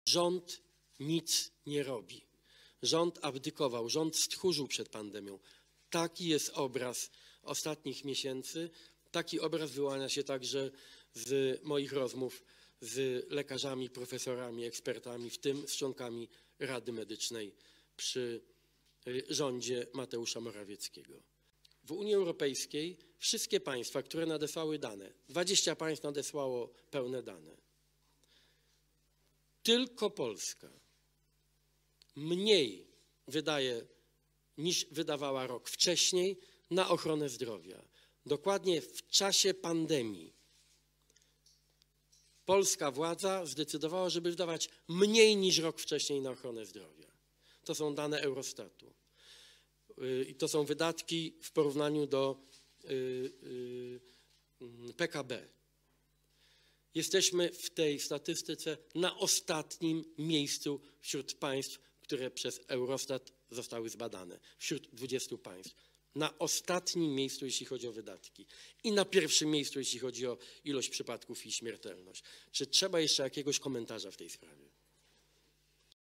Donald Tusk: Rząd nic nie robi z pandemią. Stchórzył.
Rząd abdykował, rząd stchórzył przed pandemią. Taki jest obraz ostatnich miesięcy.